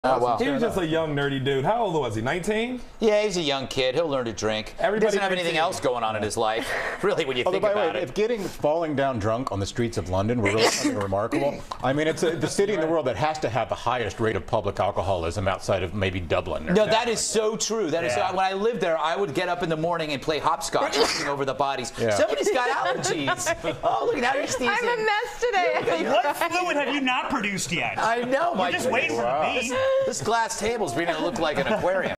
Diane macedo sneezing on fox.
Diane macedo sneezing twice on live tv.